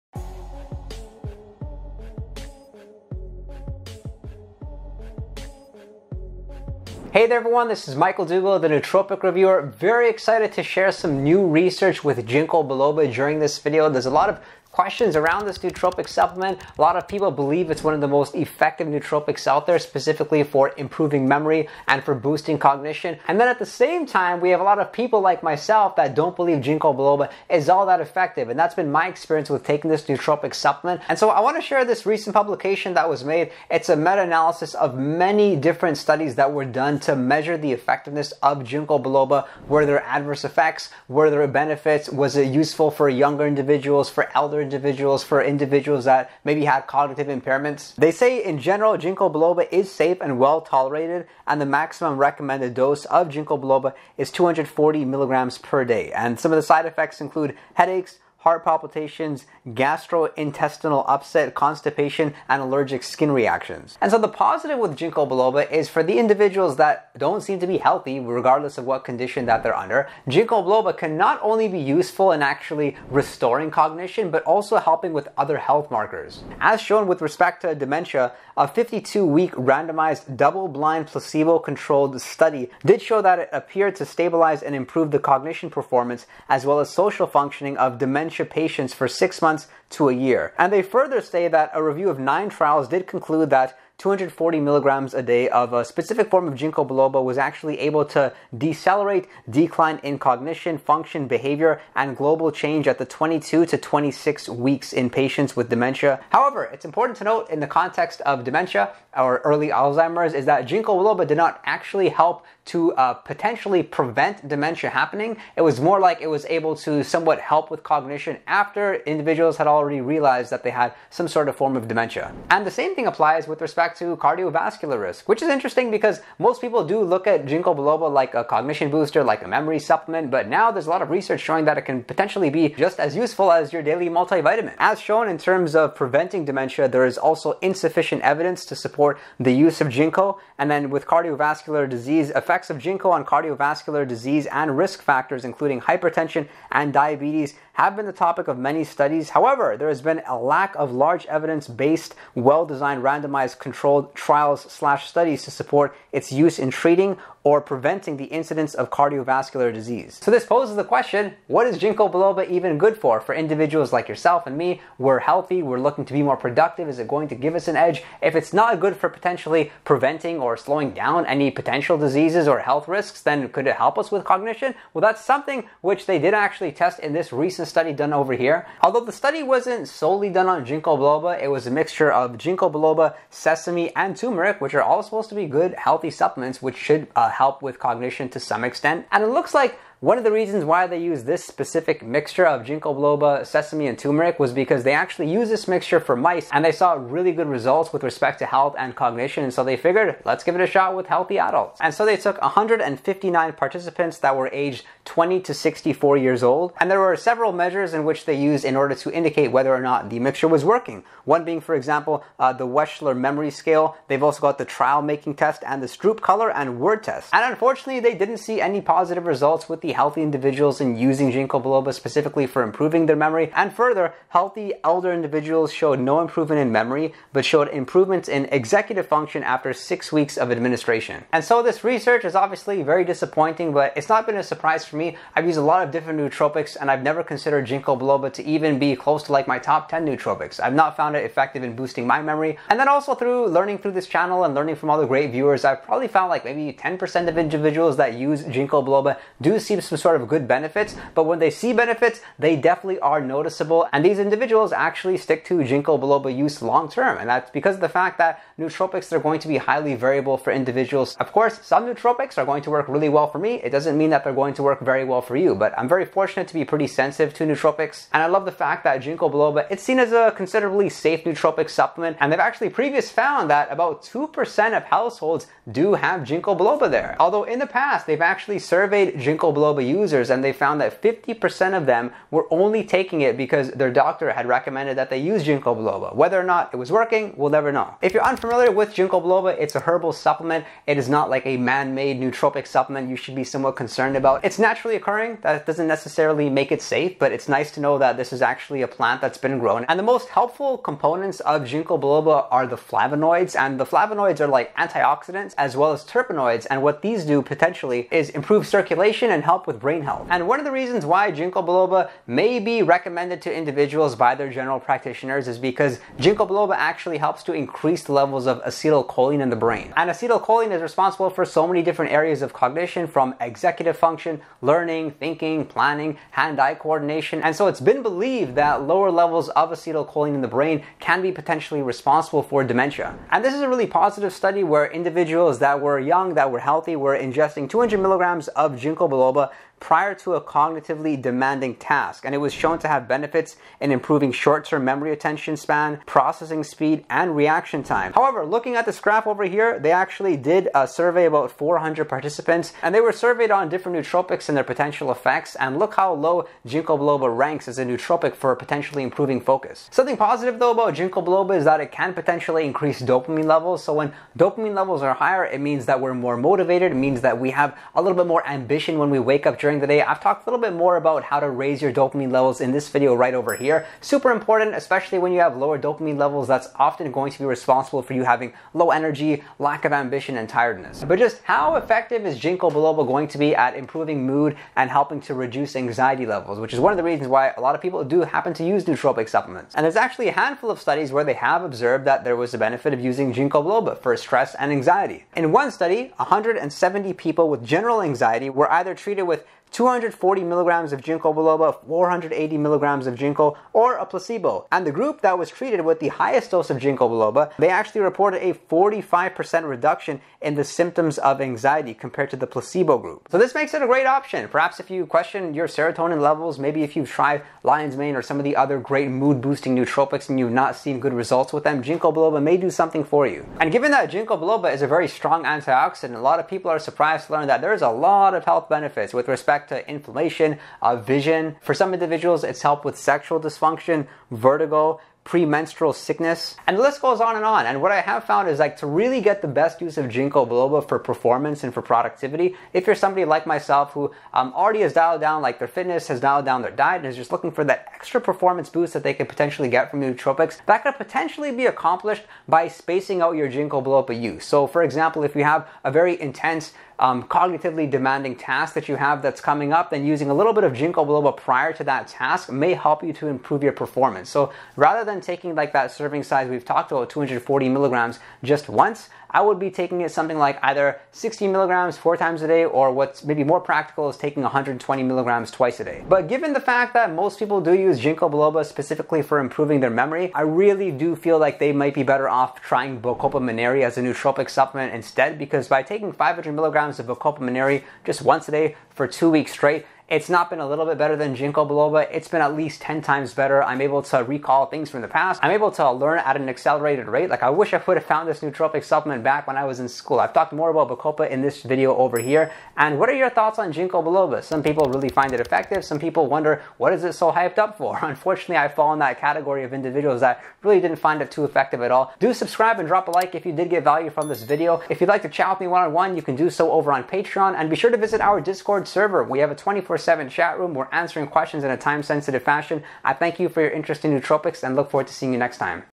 Ginkgo Biloba Benefits | New Research
Is Ginkgo Biloba the new best nootropic for improving memory and overall cognition function according to research? Here’s a comprehensive review of recent studies examining its effectiveness, specifically delving into side effects, benefits, and its influence on different age groups and cognitively impaired individuals. Stay informed about the latest research findings on Ginkgo Biloba for a well-rounded understanding of its cognitive effects.

⏰TIMESTAMPS⏰
00:00 Introduction
02:40 What Is Ginkgo Biloba Good For?
04:49 Healthy Components Of Ginkgo Biloba
06:23 How Effective Is Ginkgo For Mood And Anxiety?

Onnit
➡️ Try a FREE 15-day supply of Alpha BRAIN (VALID in USA only)